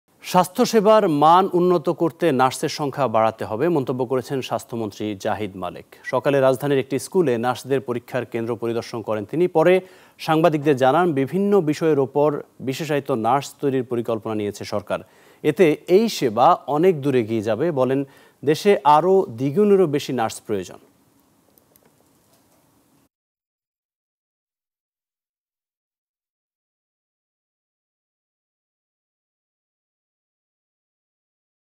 ‘স্বাস্থ্যসেবার মান উন্নত করতে নার্সের সংখ্যা বাড়াতে হবে’ | Health | Nurse | Nagorik News
‘স্বাস্থ্যসেবার মান উন্নত করতে নার্সের সংখ্যা বাড়াতে হবে’